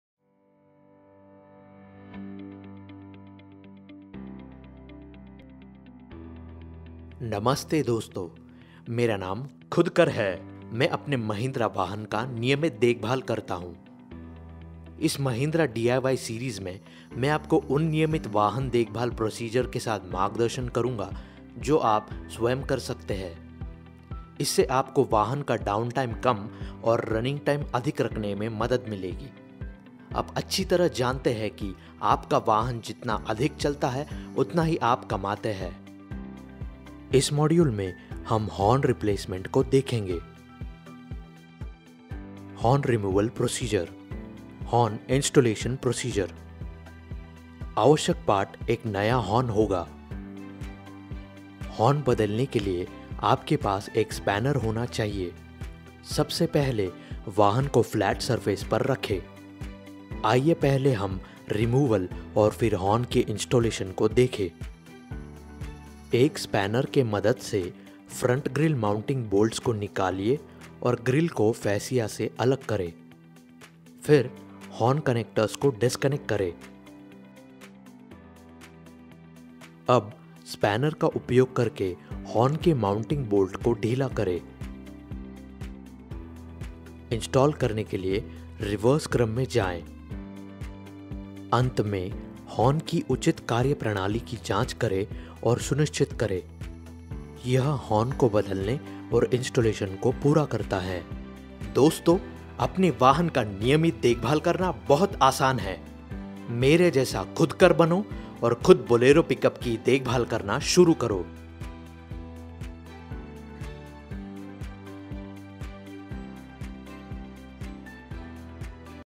Horn Replacement | Bolero PIK-UP (Hindi)
Film on horn replacement showing two stages
1) Horn removal and
2) New horn installation